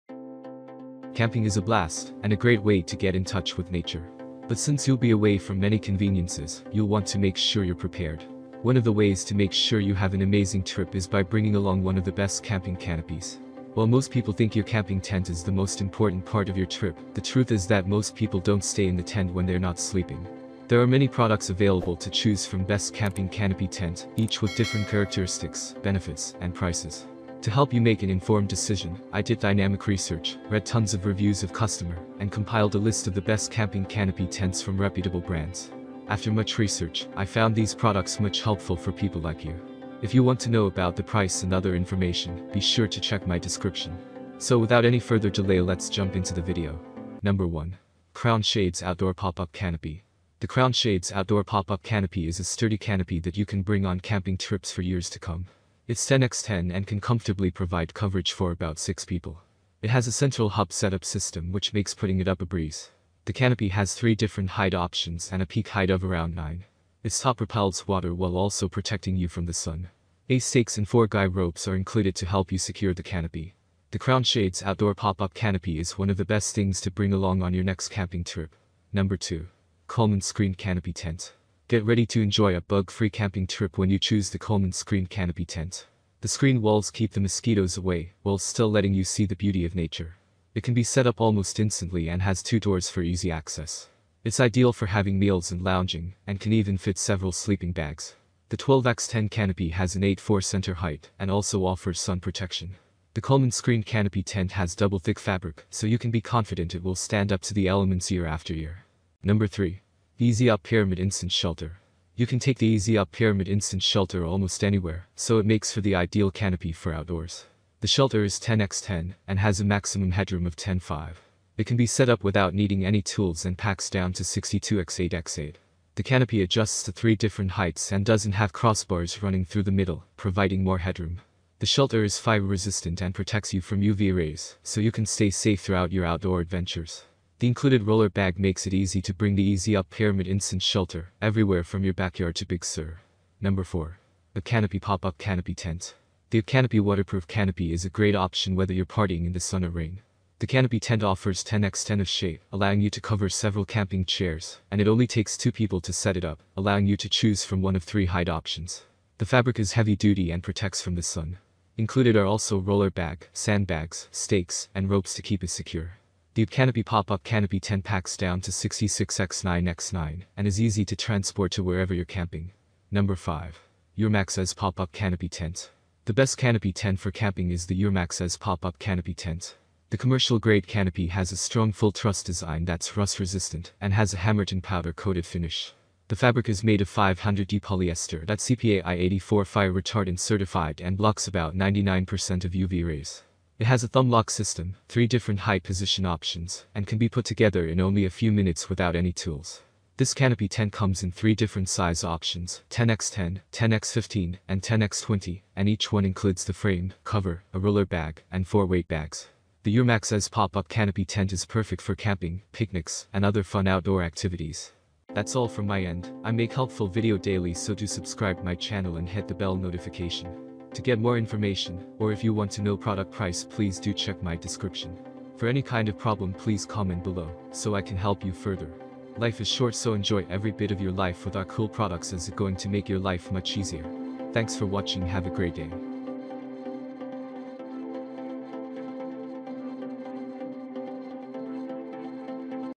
Best Camping Canopy Tent On Amazon 2023 (Buying Guide)
✔️Best Camping Canopy Tent 🔥 💥
📌Product Link📌
✅ My Picks ✅1.  Crown Shades Outdoor Pop-up Canopy
✅ Best Value ✅2.   Coleman Screened Canopy Tent
✅ For Beginners ✅3.  E-Z Up Pyramid Instant Shelter
✅ Premium ✅4.  ABCCANOPY Pop up Canopy Tent
✅ Trending ✅5.  Eurmax Ez Pop Up Canopy Tent